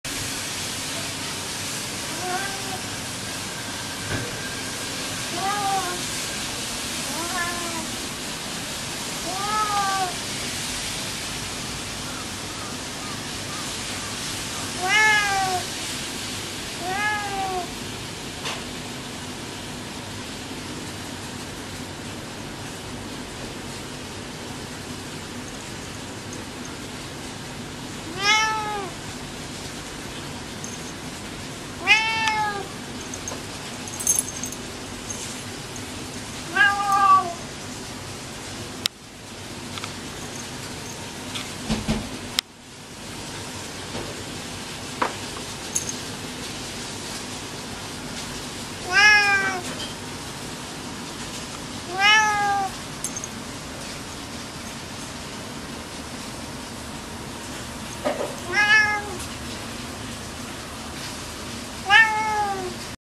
Mimi on the tower
She does this strange reaction when she wants to play